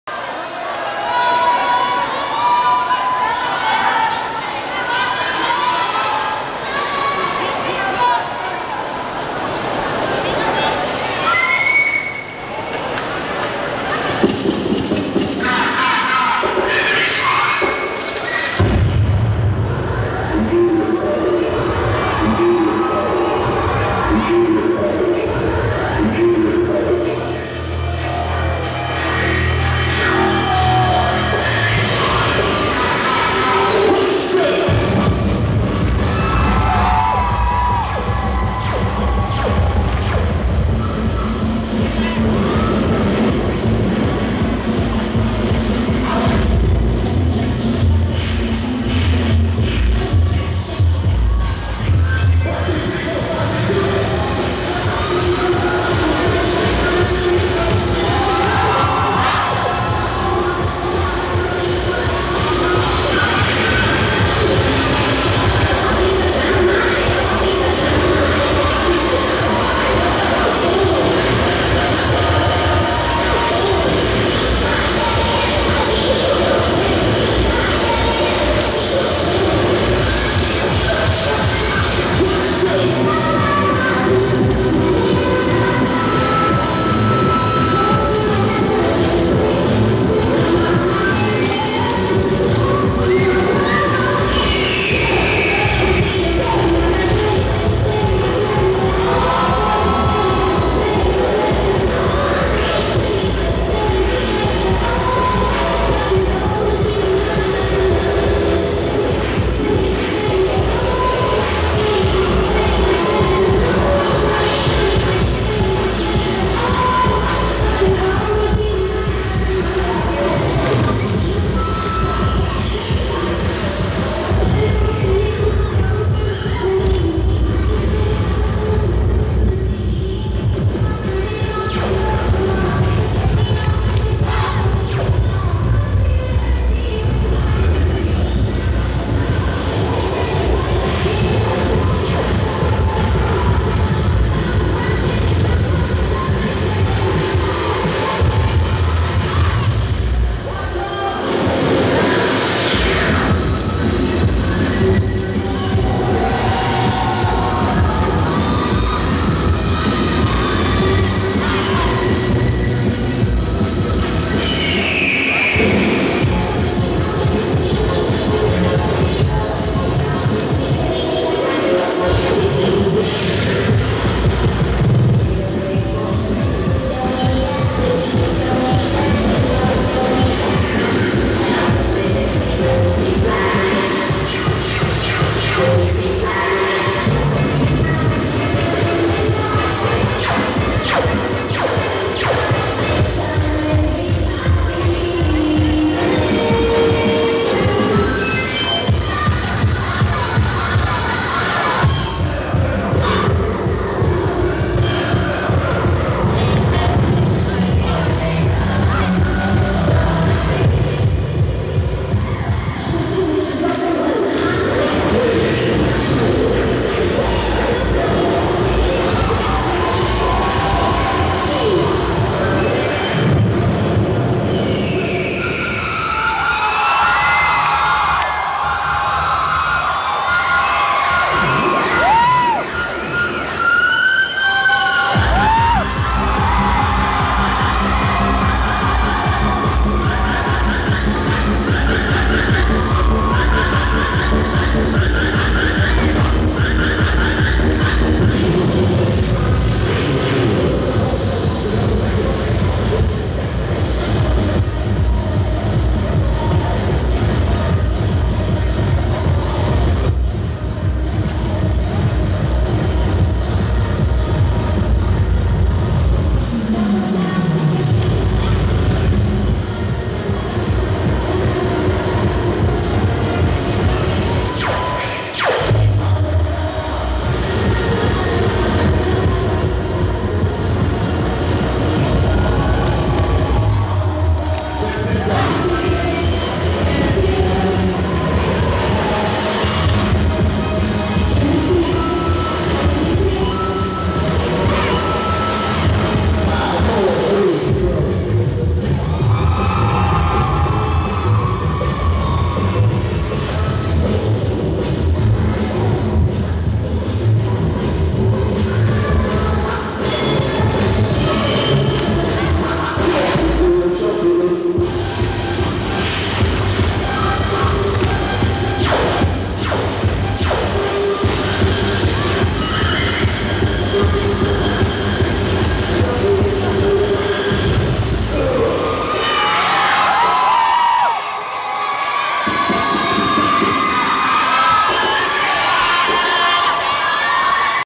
Medina College-senior's cheerdance
My cousin, Edisah's college cheerdance.